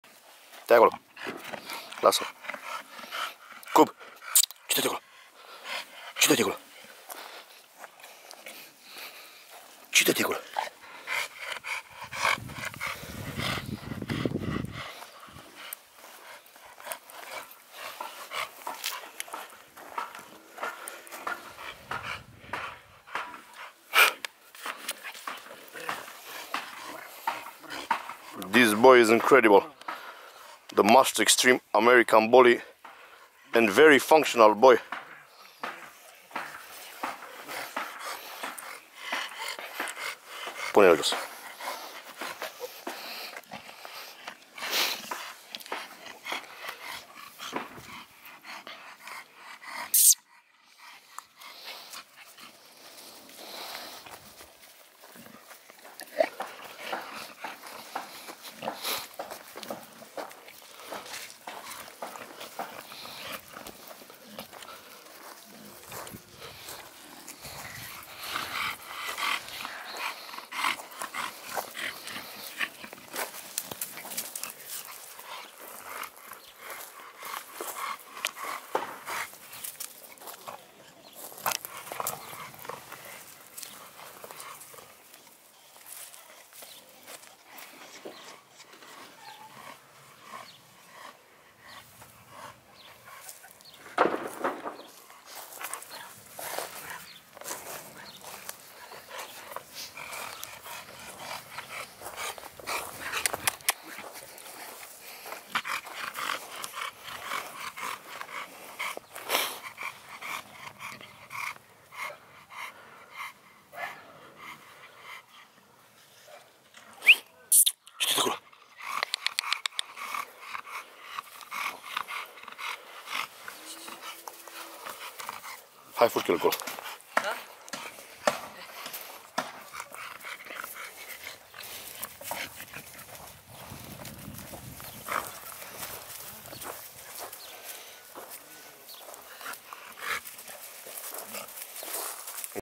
MR.CUB VERY EXTREME AMERICAN BULLY 17 MONTHS OLD .PRODUCT BIG DOGS ROMANIA KENNEL
MY BOY MR.CUB AT ONLY 17 MONTHS OLD

BLOODLINE BIG DOGS ROMANIA

BEST AMERICAN BULLY EXTREME